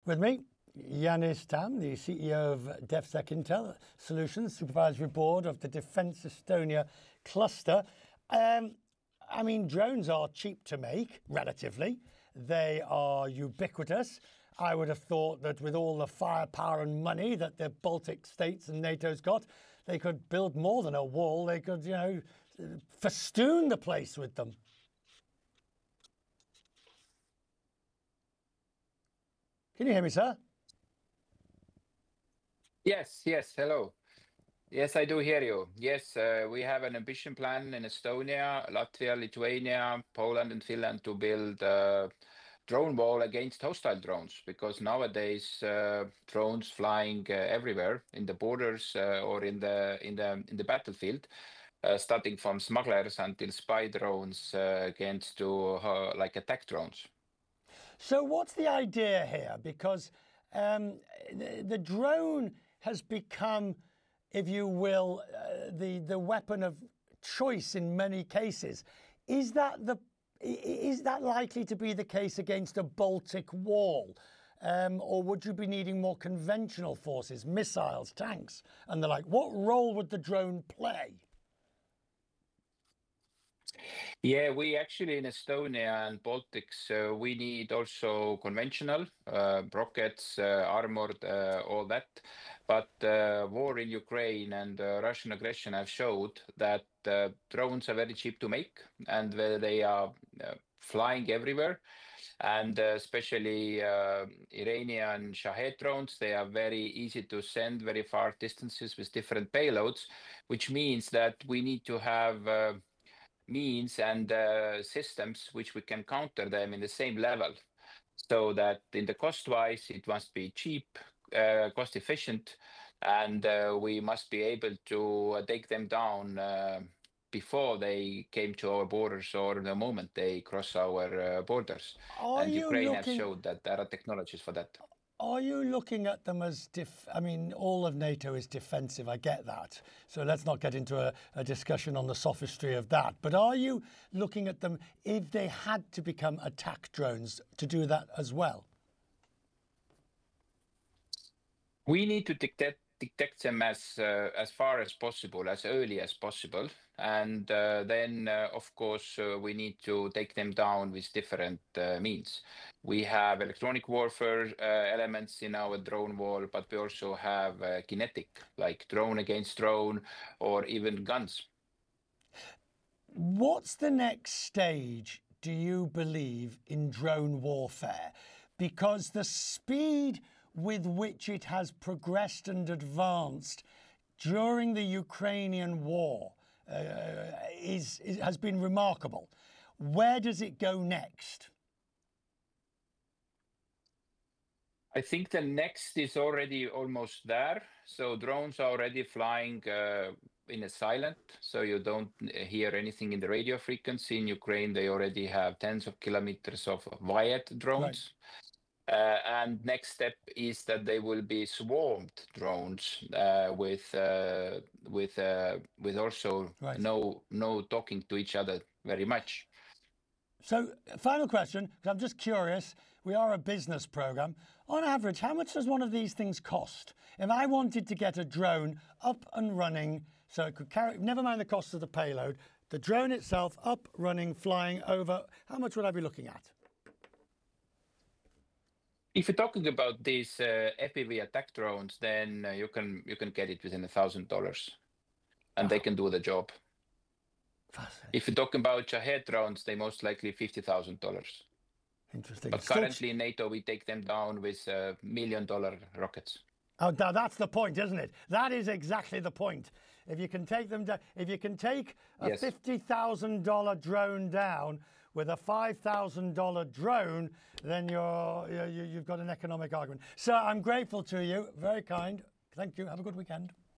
Jaanus Tamm Discusses the Capabilities of Drone Warfare
DefSecIntel Solutions CEO Jaanus Tamm discusses the capabilities of drone warfare.